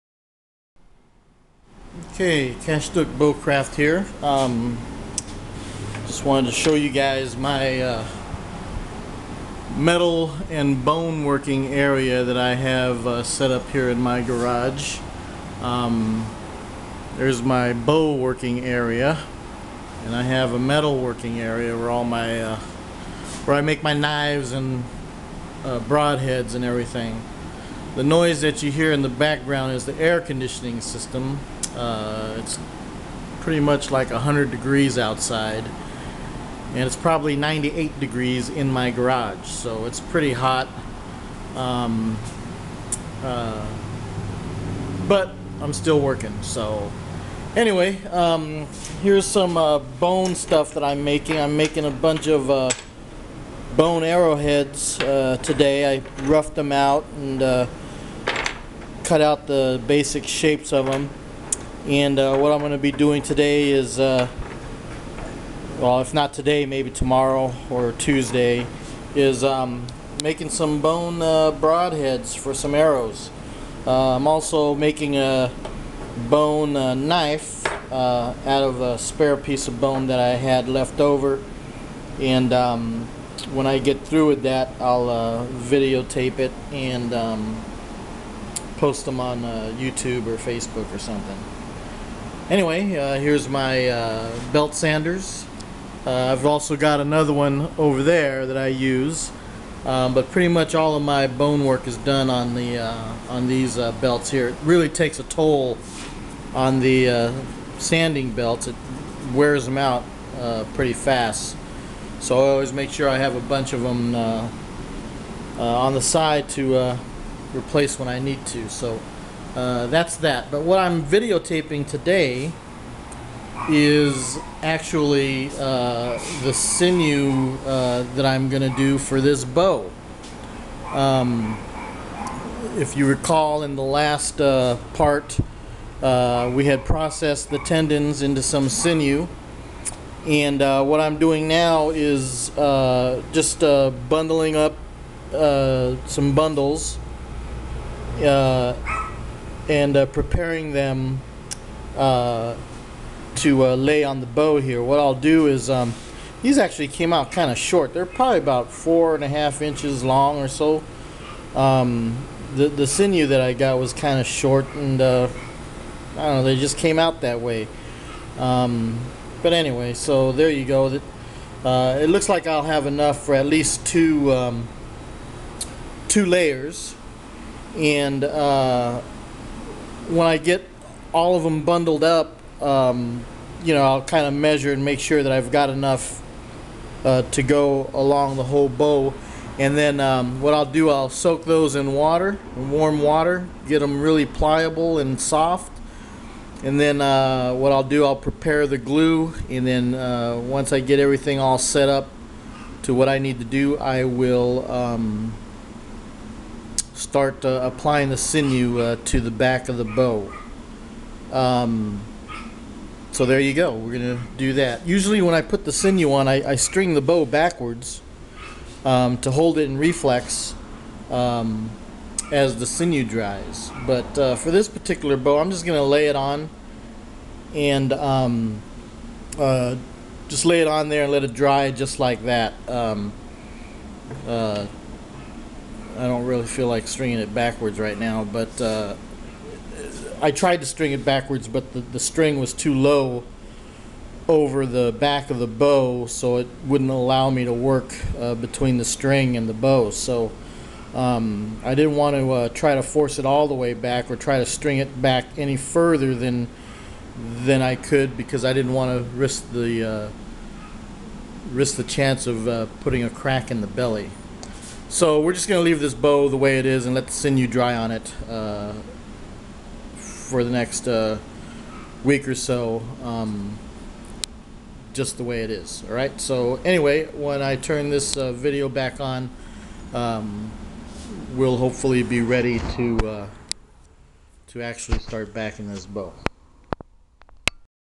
Making a Hickory Short Bow Part 7
Making sinew bundles and getting ready to make the glue.